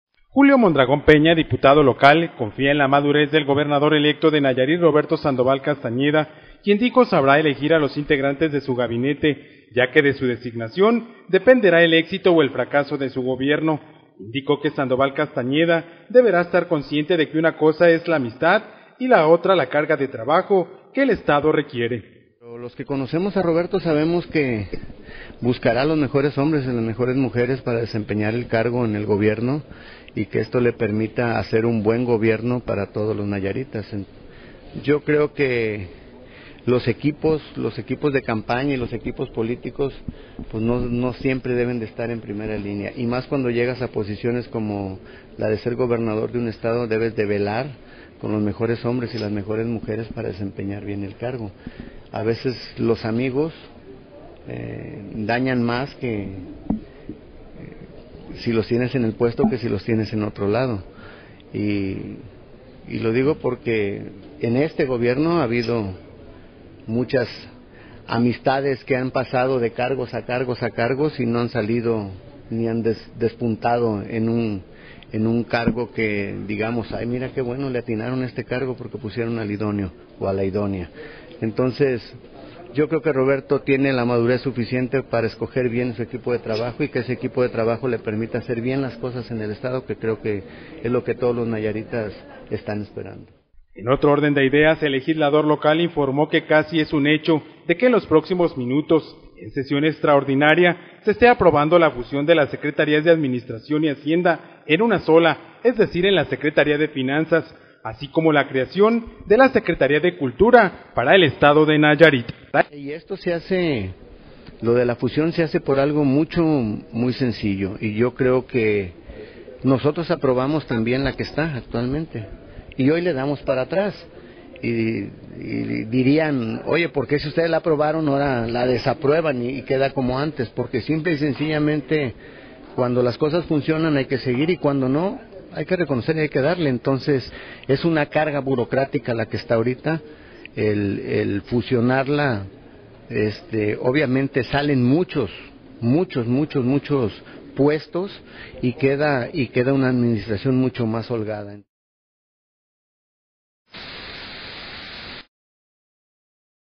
Amigos de Roberto podrían dañar su gobierno: Julio
El diputado local del PRI, Julio Mondragón Peña considera que los equipos de campaña y equipos políticos no siempre deben estar en primera línea al integrar el gabinete de un gobierno estatal, por lo que confía en la elección de Roberto Sandoval